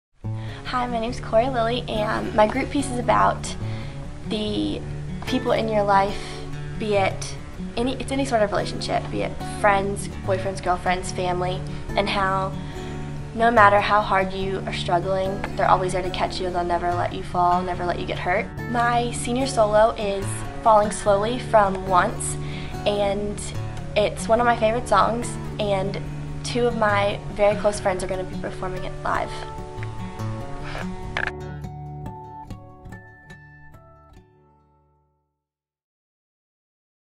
Senior Dance Concert - Corey Lilley
The senior dance majors will present the Senior Dance Concert: Lasting Impression on January 27-29, 2012.

Ohrstrom-Bryant Theatre
Shenandoah Conservatory